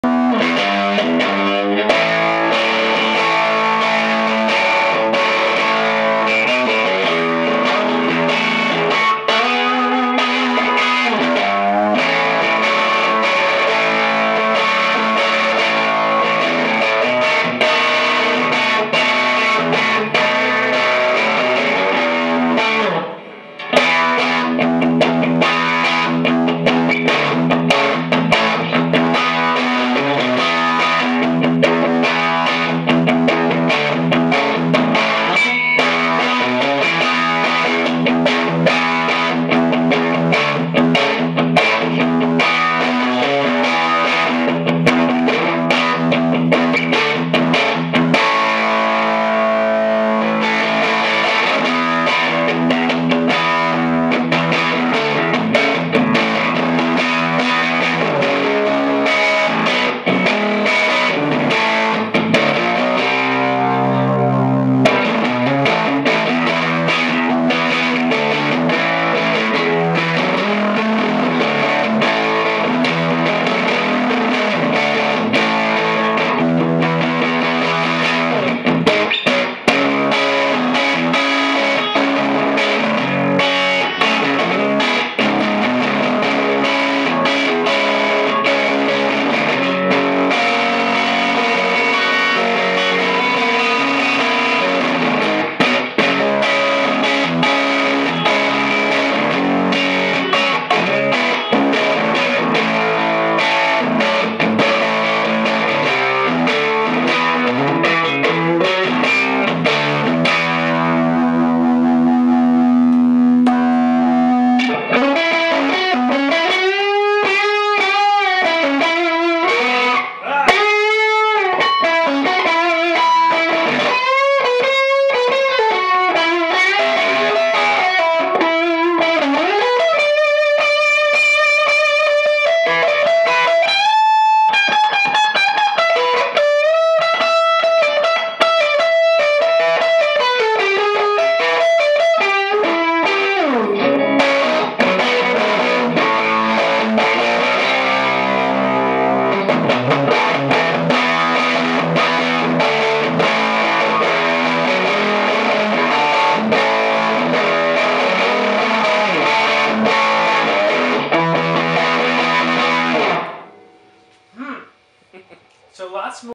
Wattkins.com Thrifty Wreck Tour Amp Demo
Wattkins.com Thrifty Wreck Tour Amp through a WGS Reaper speaker in an open back cabinet. MIM Strat with Seymour Duncan pickups. This tube amp is an "Express" circuit scaled to about 10 watts output. Developed in the "Tone Per Buck Club" at the wattkins.com amp forum. The video is dreadful (it didn't occur to me that the camera would focus on what is closest to it rather than what is in the center of the frame) but the audio is quite good and the audio is all that really matters here. The pedal he kicks on near the end is an RF Drive -also a wattkins.com project.